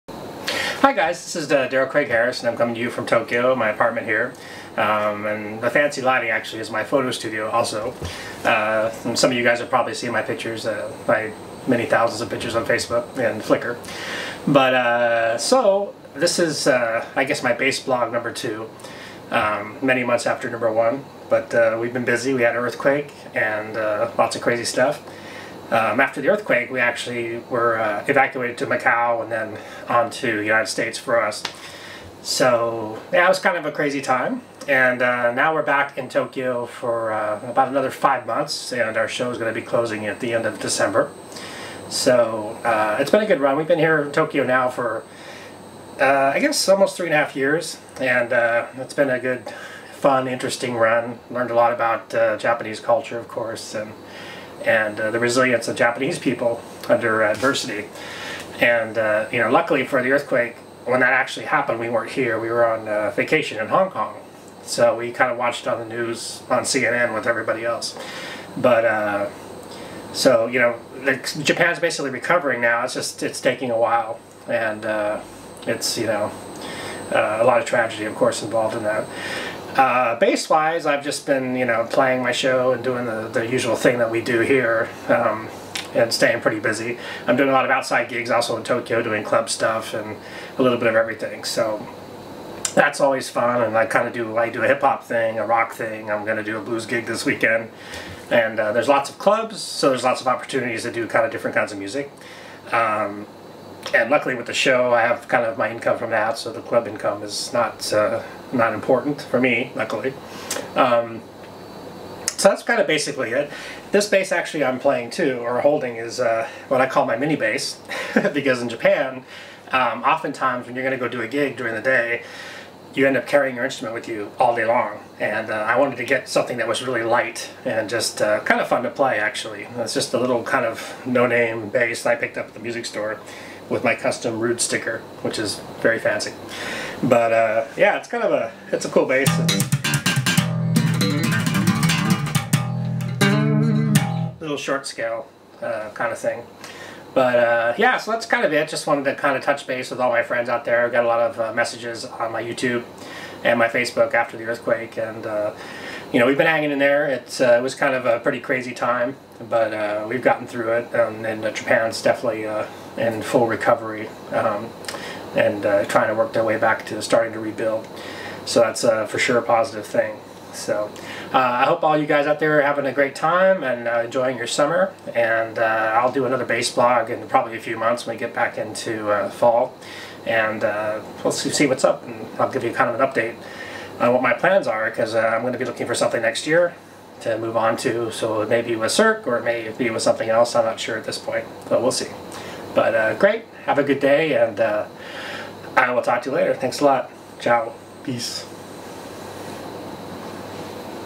Tokyo bass blog #2 - Cirque du Soleil ZED Bassist Darrell Craig Harris in Tokyo
My second video from my apartment in Tokyo discussing our life as a musician with Cirque du Soleil in Japan